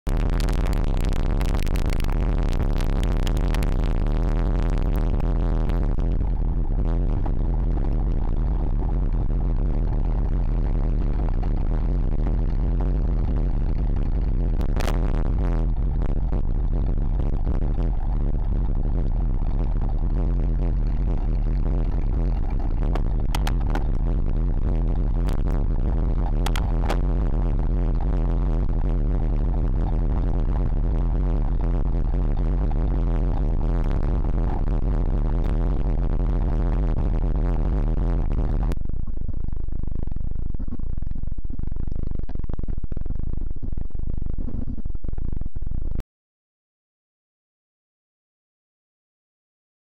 Automobile on Rails to Trails path 2018-09-14
Vehicle is looking for a shortcut.